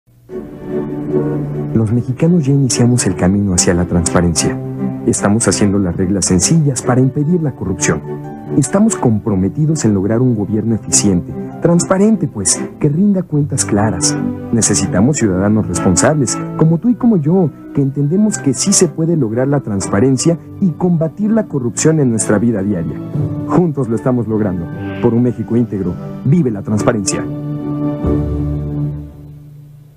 Comerciales mexicanos: SECODAM 2002 2
Campaña por un México mejor ya no mas mordidas que sirve para acabar con la corrupción regresando al tema de SECODAM actualmente es SFP